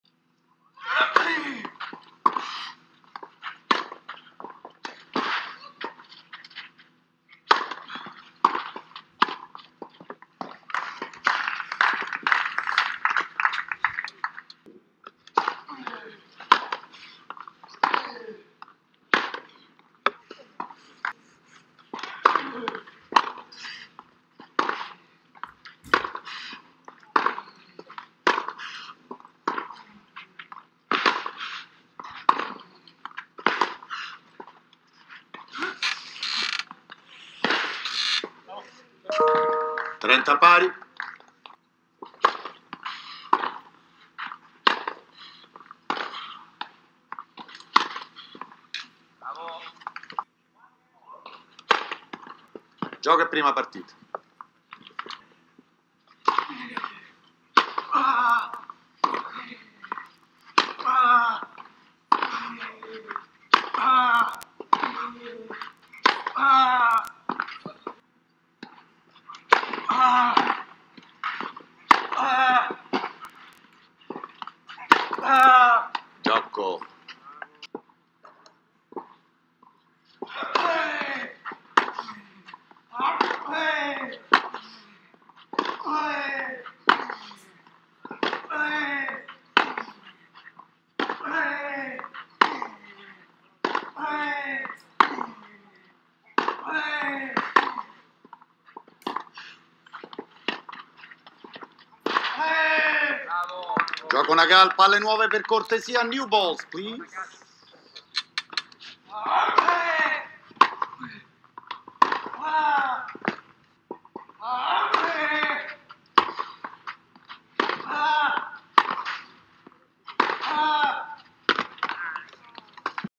Sumit Nagal vs Giulio Zeppieri - ATP Parma Challenger Round 1 Highlights
Sumit won the match against the former world junior rank #12 Zeppieri 6-4 6-4